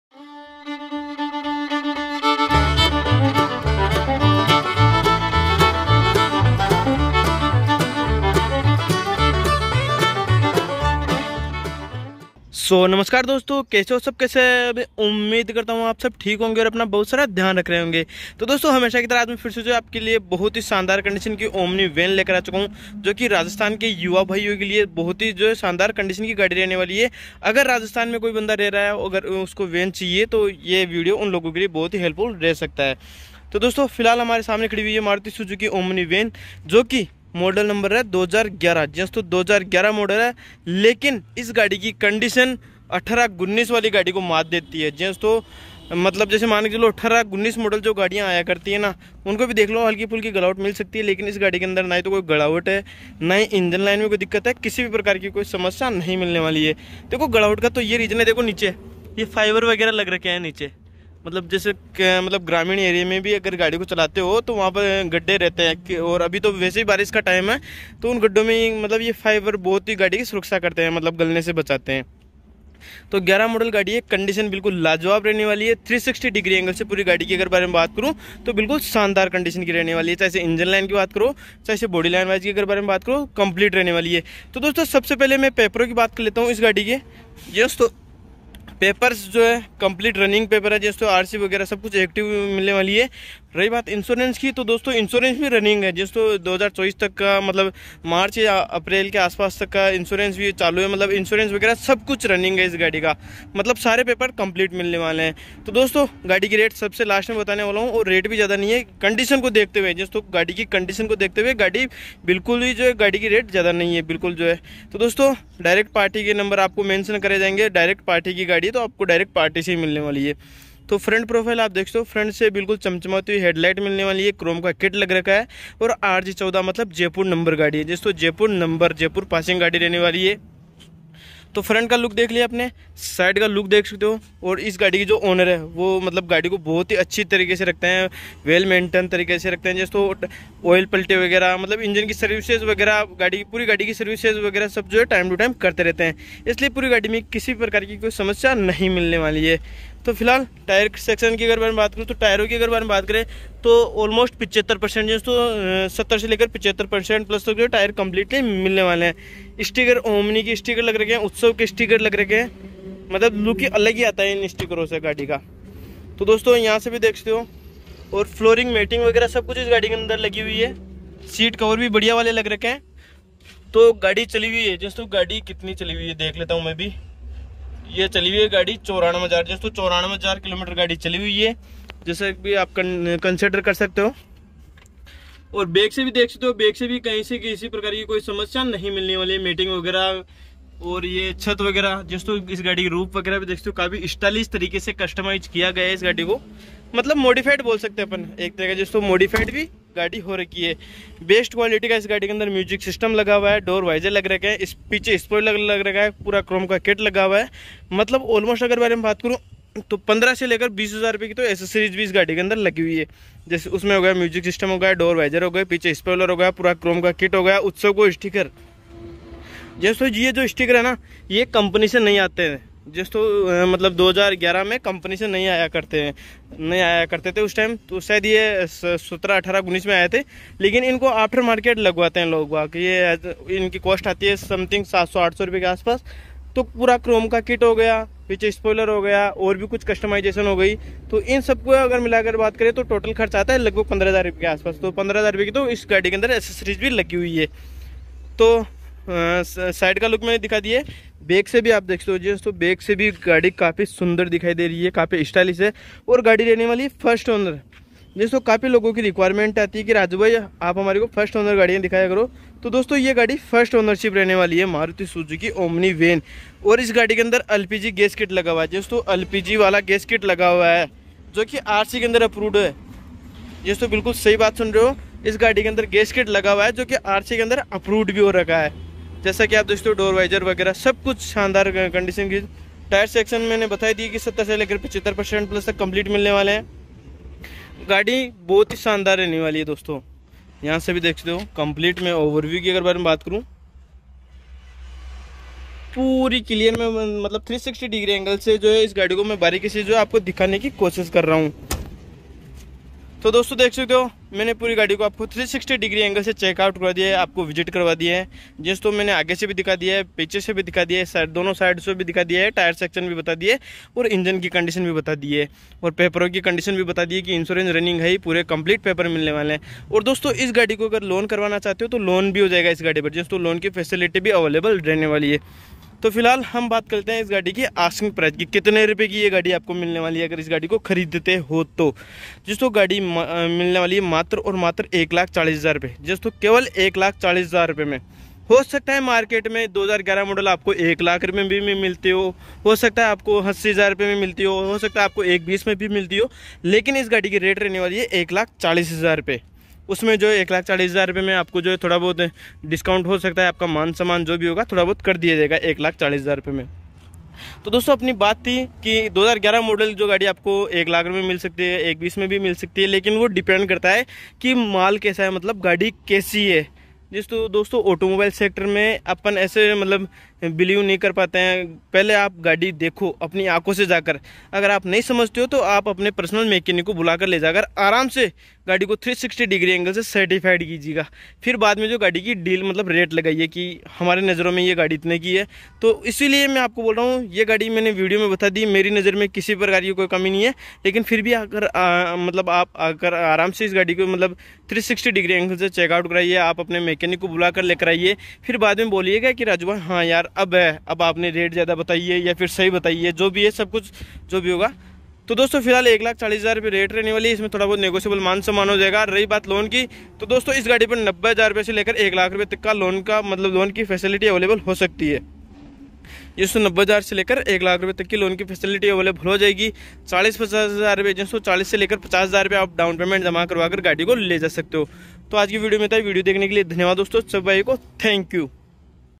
2011-Model Maruti Suzuki Omni Van खरीदें - मात्र 40 हज़ार देकर | Used Omni Van For Sale
Address;- Main Jaipur-Ajmer Road, Laddi Hotel Ke Pash, Dudu
Contact Only for Rajasthan Person.